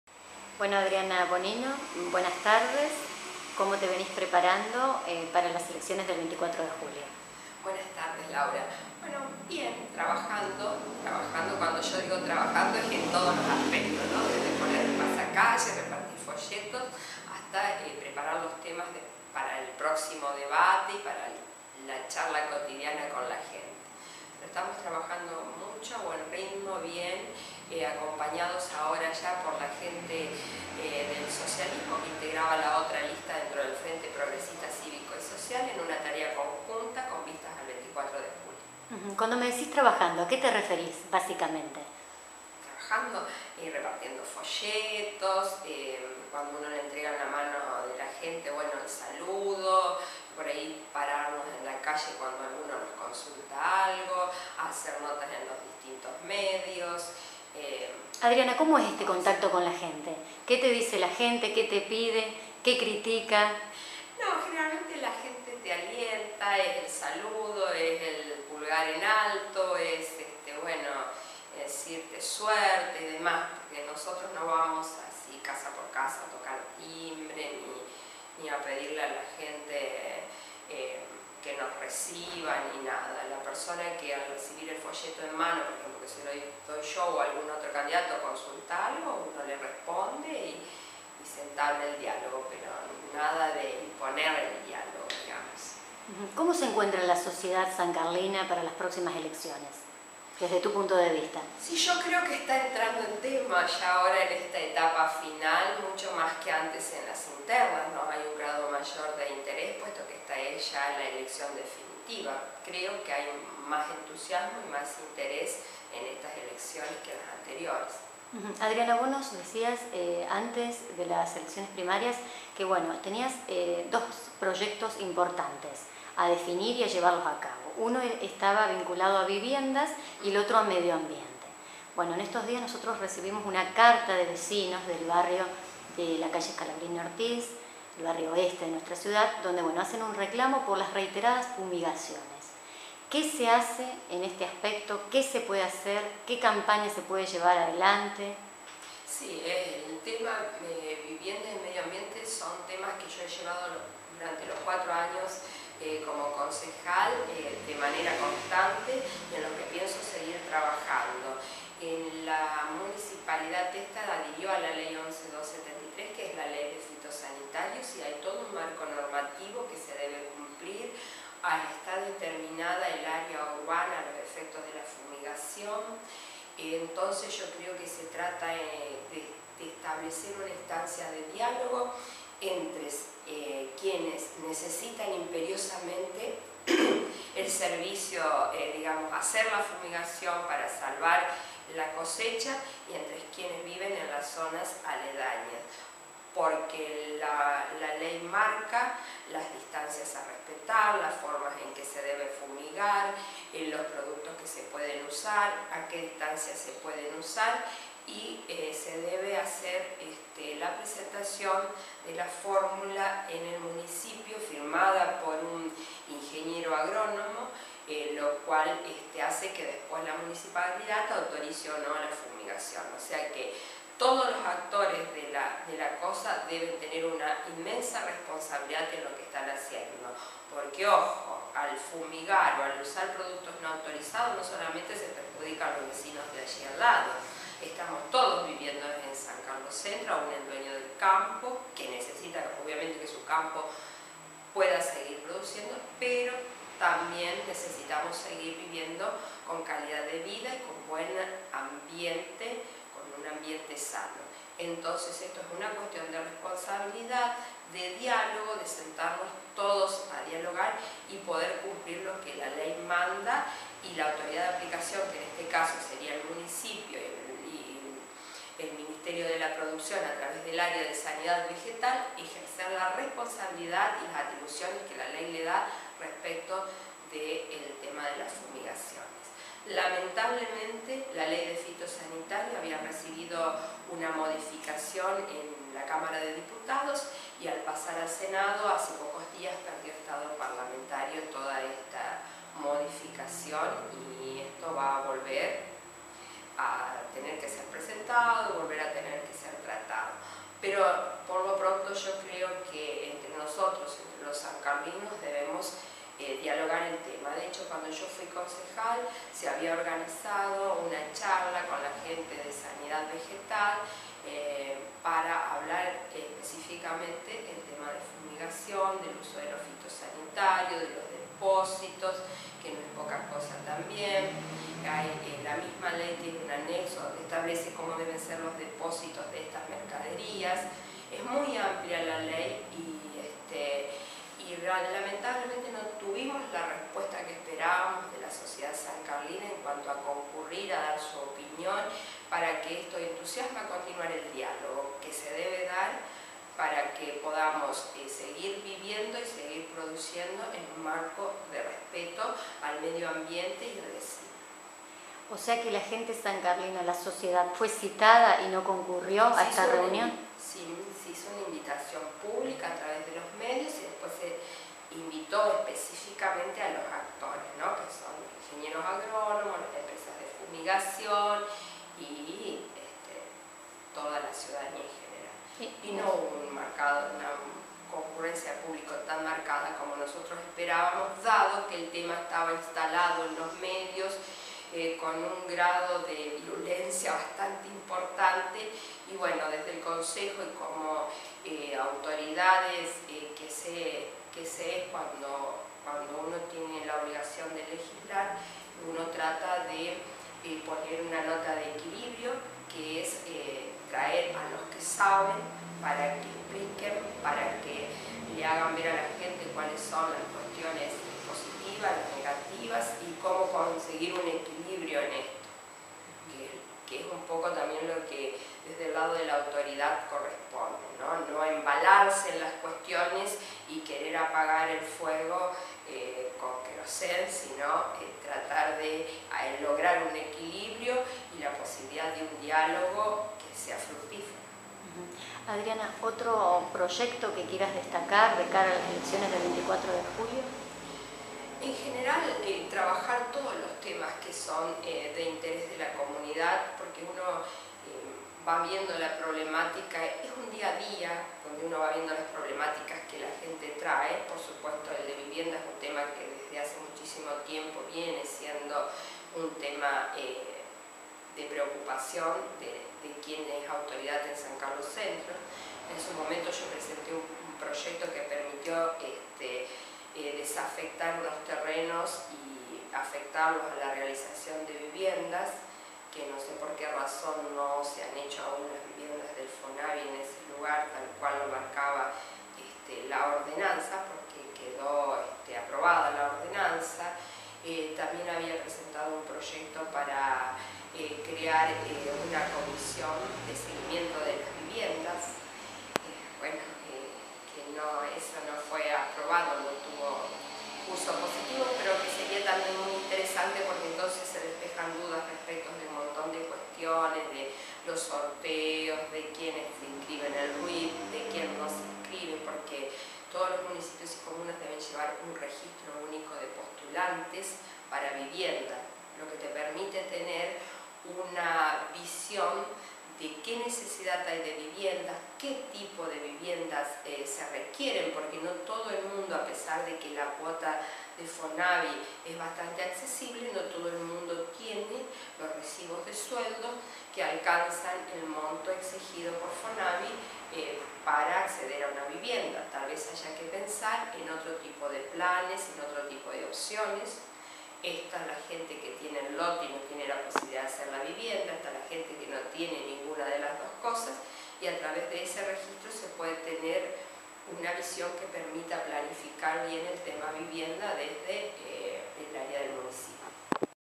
Adriana Bonino de cara a las elecciones del 24 de julio
La candidata a Concejal, por el Frente Progresista Cívico y Social, Adriana Bonino, comentó en una entrevista con "El Urbano", aspectos referidos a los diversos  trabajos que viene realizando, los que se encuadran dentro de la campaña política de cara a las elecciones generales del 24 de julio, como así también destacó consideraciones  de sus propuestas en materia de vivienda y medio ambiente.